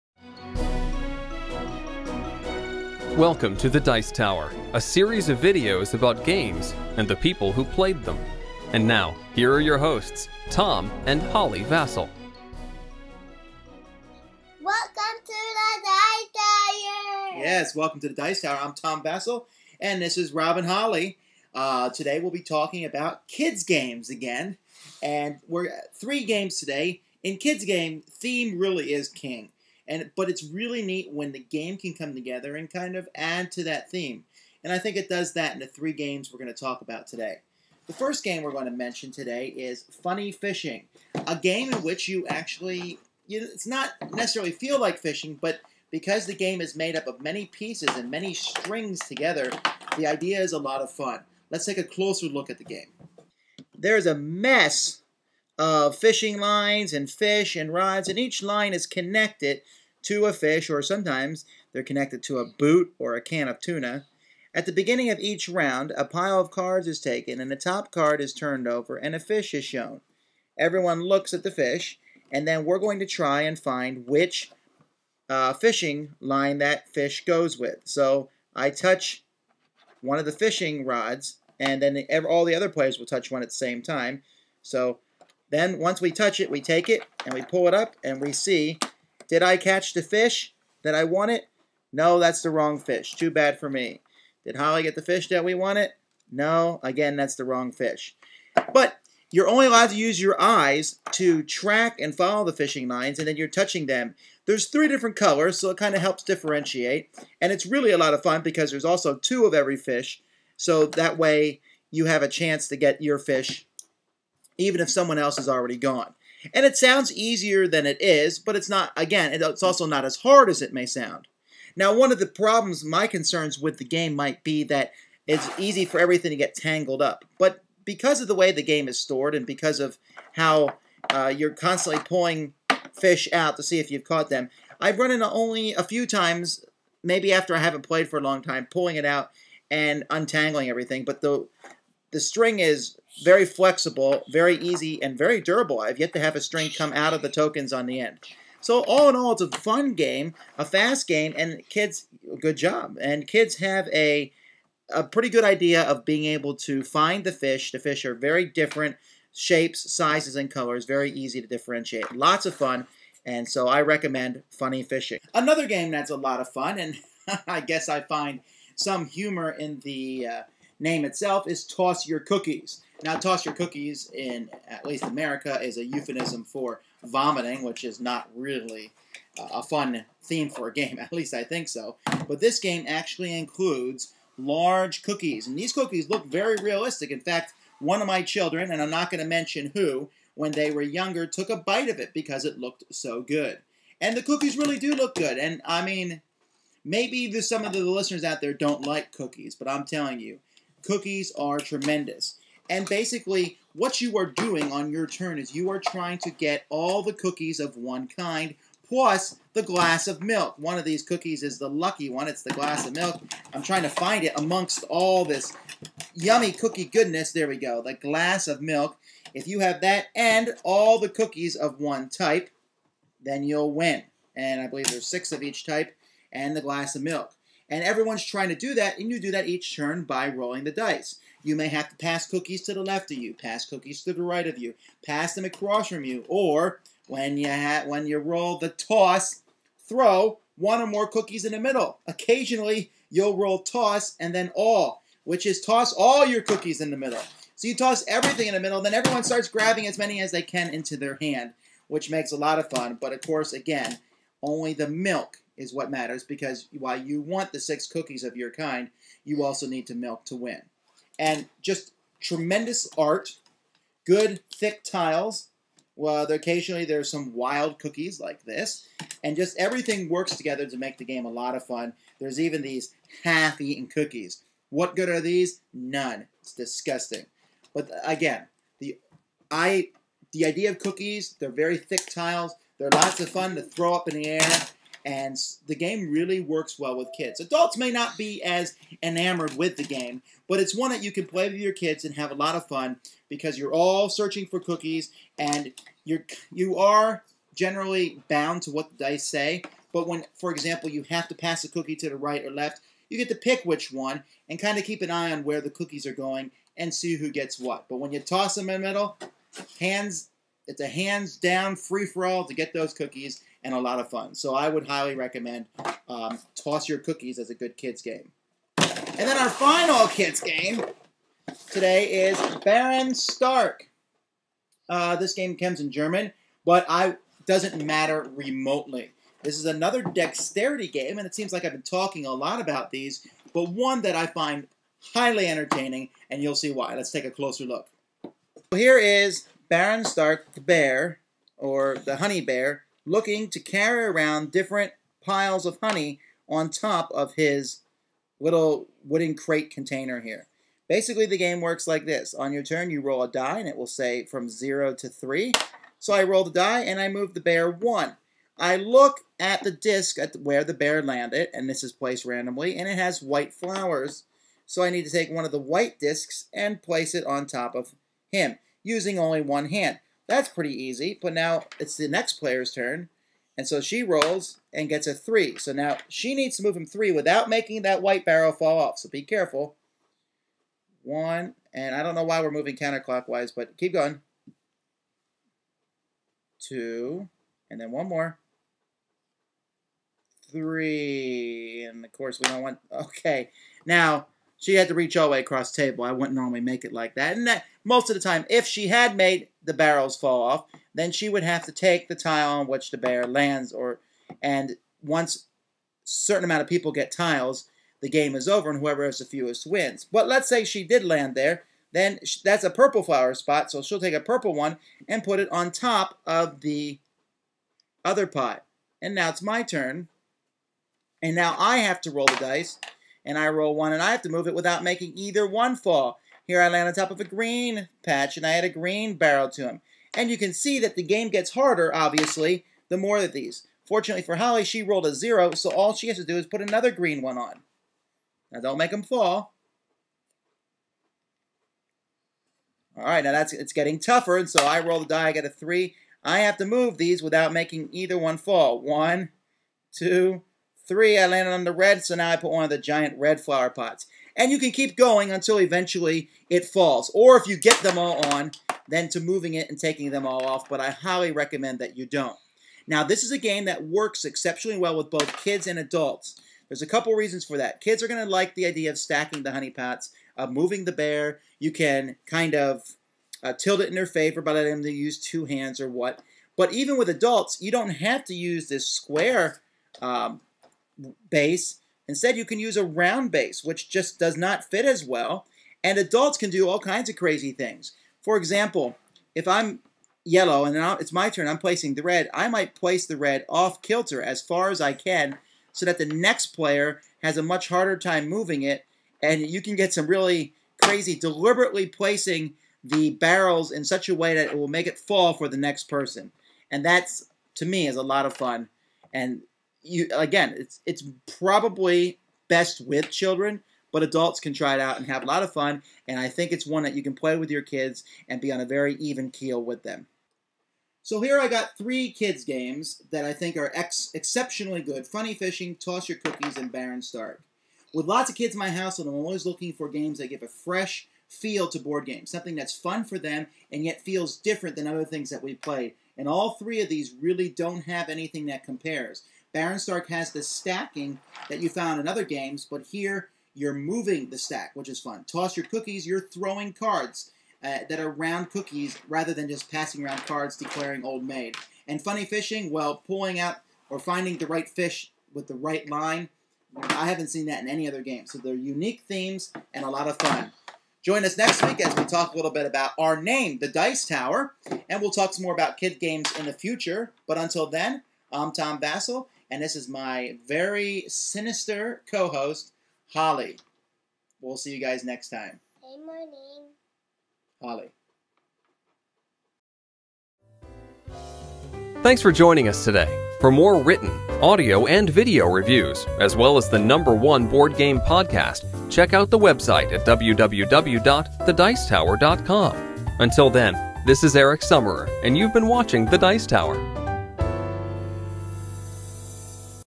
DTV009 - Kids Games C - with Tom and Holly Vasel
Tom and Holly Vasel talk about some great kids games.

00:00 - Introduction
00:39 - Funny Fishing
02:45 - Toss Your Cookies
05:20 - Baren Stark!